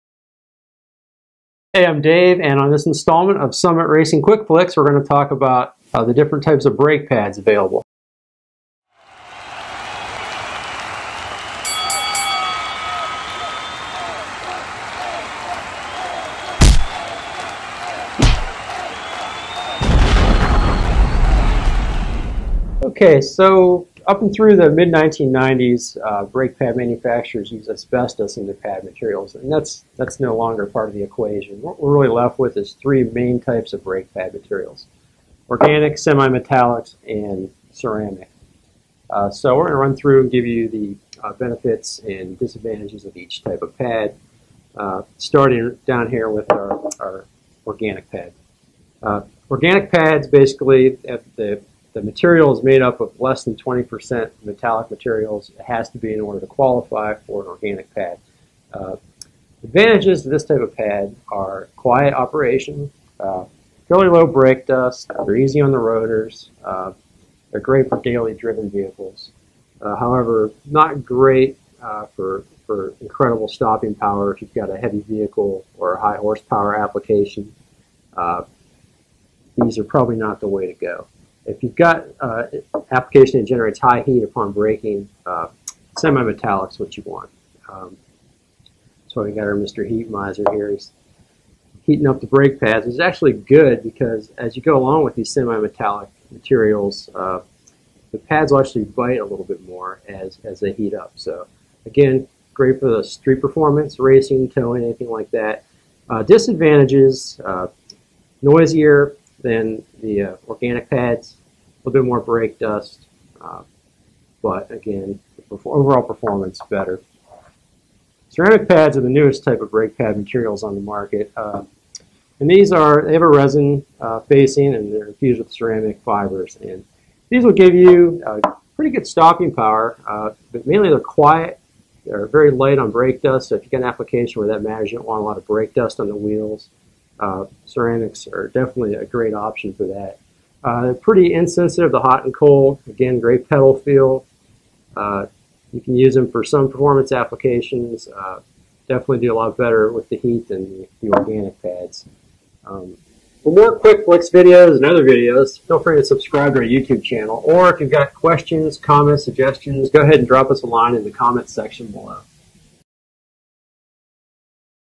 Ceramic vs Semi Metallic Brake Pads - Summit Racing Quick Flicks
Learn the difference between ceramic, semi metallic and organic brake pads. Dave explains which brake pad performs best, makes the least amount of noise, leaves the least amount of brake dust, and which shoes will last the longest.